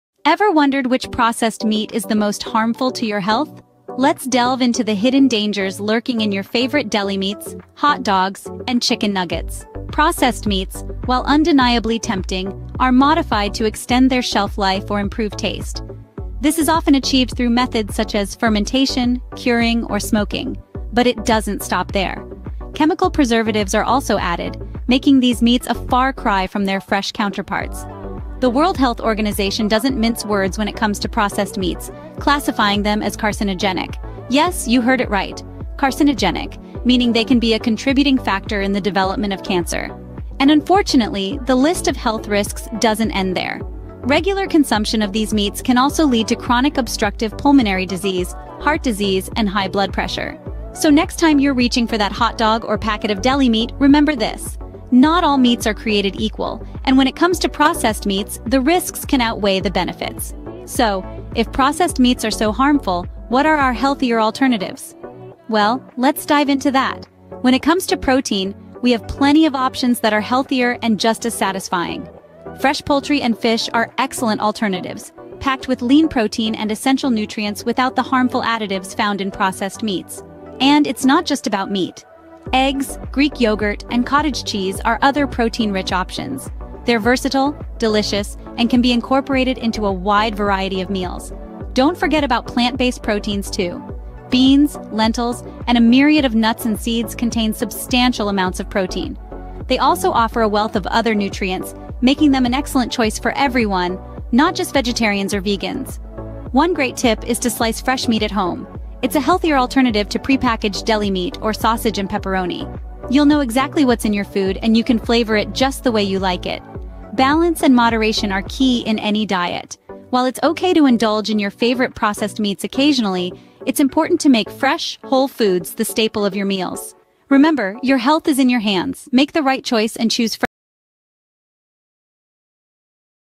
⌈Healthy Living⌉ The hot dogs, the deli meat, the chicken—which processed meat is the most harmful?
Meat has some well-known health benefits, including a high supply of fatty acids and minerals such as iron, zinc, and B vitamins. However, not all meats are created equal. Red meat, for example, has the advantage of being a high-quality protein source; yet, if ingested in excess, it has been linked to type 2 diabetes and heart disease.